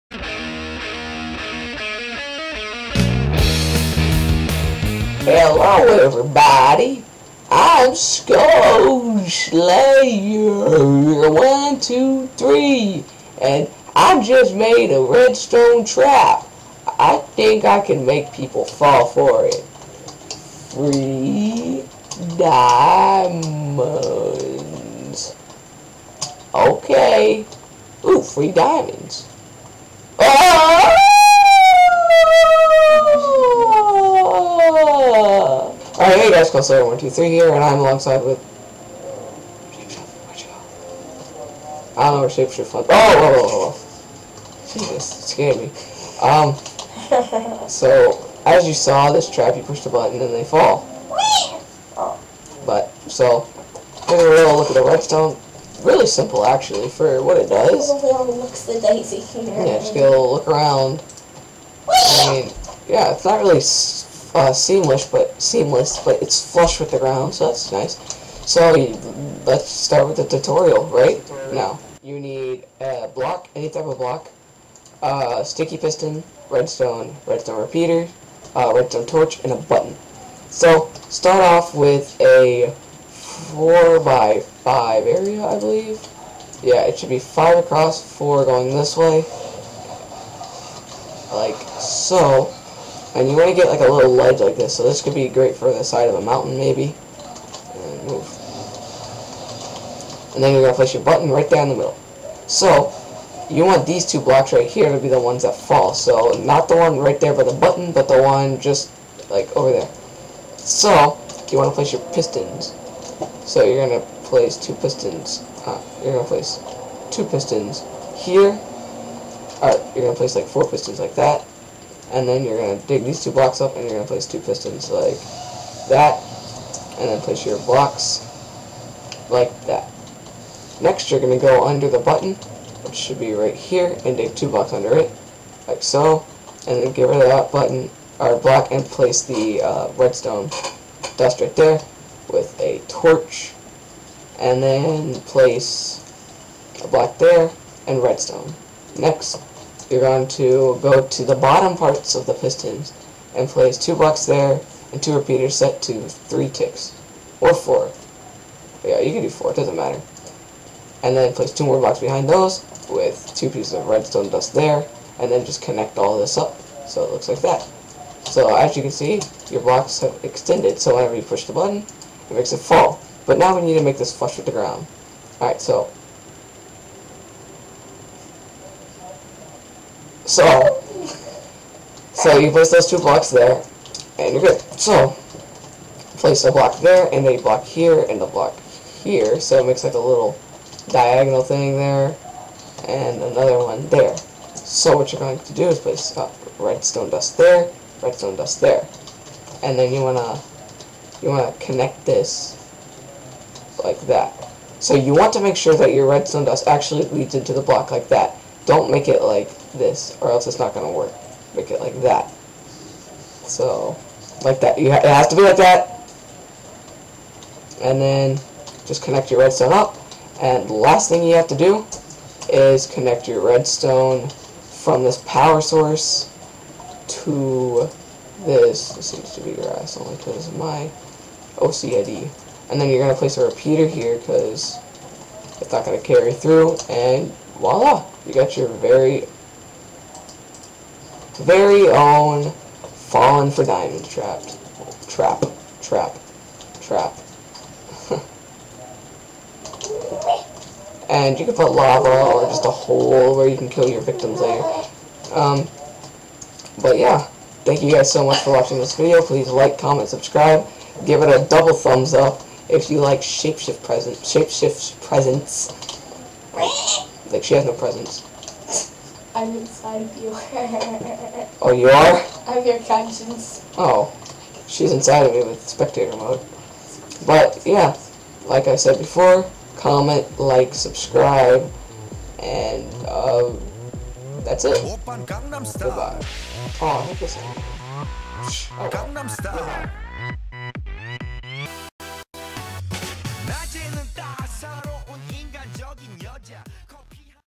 Redstone Creation #3| FALLING FOR DIAMONDS
The third Redstone Creation! Comment if you want more skits in the beginning!
p.s. i own no music